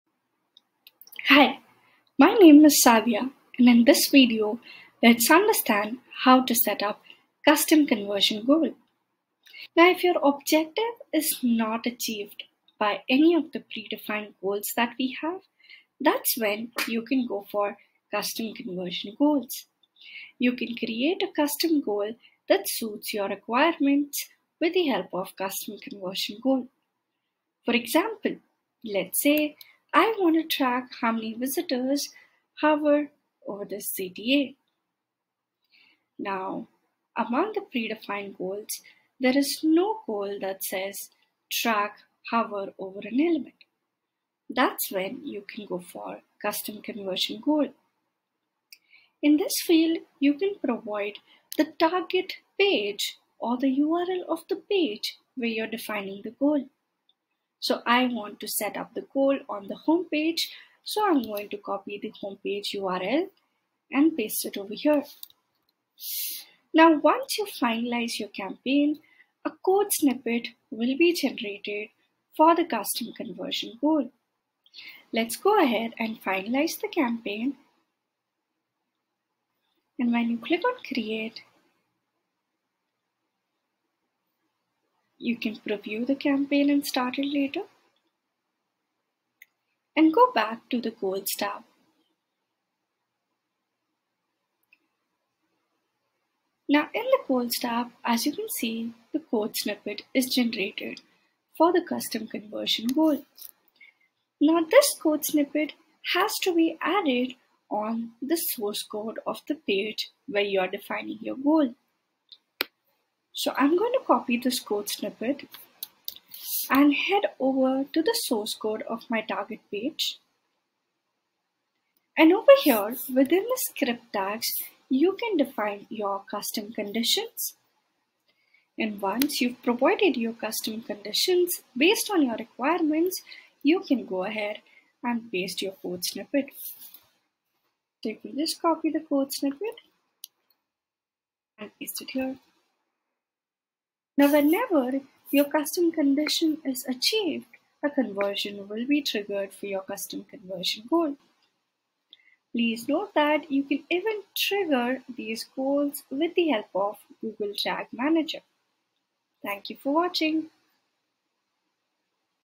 Goals | Custom Conversion goal
The Tracks Custom Conversion goal in VWO enables you to define a custom goal. The custom conversion goals typically don't deal with button clicks, link clicks, etc.

For example, if your goal is to track when someone hovers on an element, you can create a custom conversion goal to trigger the hover event. Basically, you get a customizable goal snippet that you can associate with any event, add any custom conditions to, or map with any activity that VWO doesn't track out of the box.